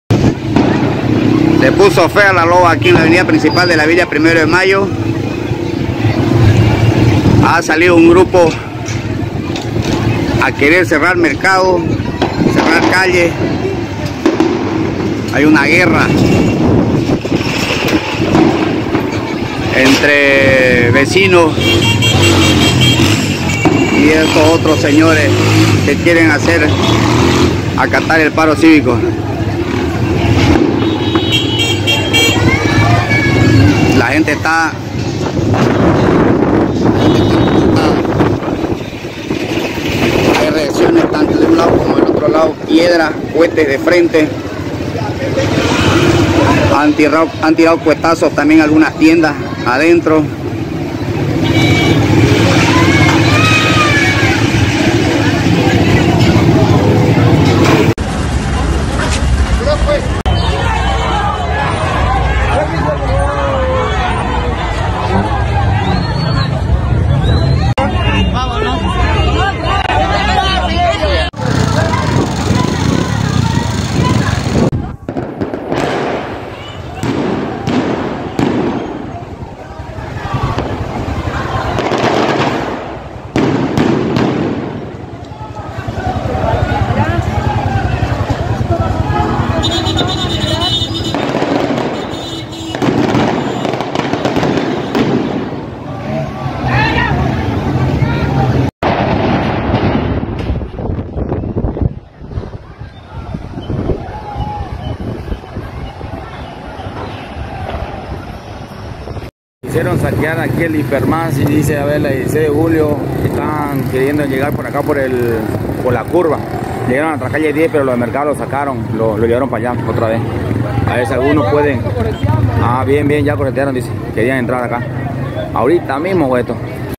EFRENTAMIENTOS EN LA VILLA 1RO DE MAYO 22 DE NOVIEMBRE
Esta mañana se registraron enfrentamiento entre comerciantes de la zona del mercado central de la Villa 1ro de Mayo. Un grupo de personas quienes llegaron en motocicletas (algunos encapuchados) exigiendo el cierre de los puestos de ventas del mercado central Coarco, los venteros de la calle 10 y los que abren sus comercios sobre la av. principal. Tumbaron alimentos al suelo con violencia sembraron terror a comerciantes y transeúntes, situación que llevo a enfrentamientos de propios y extraños además de generar  opiniones divididas sobre el cierre o no cierre de los comercios hoy martes 22 de nov.
Hubieron desmanes y excesos por parte de este grupo de personas, algunos heridos y sobre todo mucho temor en las mujeres tanto que venden como las que compran en esta zona donde llegan de todos los barrios a abastecerse.